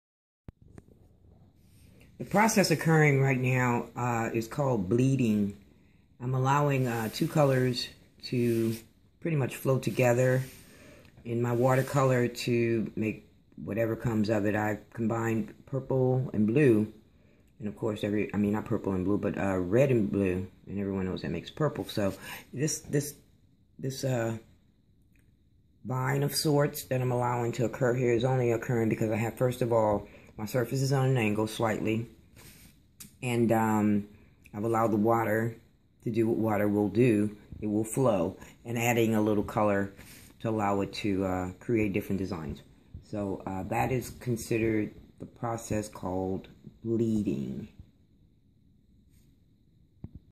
Impressionism by Impressionist FineArtist TuckerDe
Impressionist FineArtist TuckerDemps explain the watercolors technique called " Bleeding ".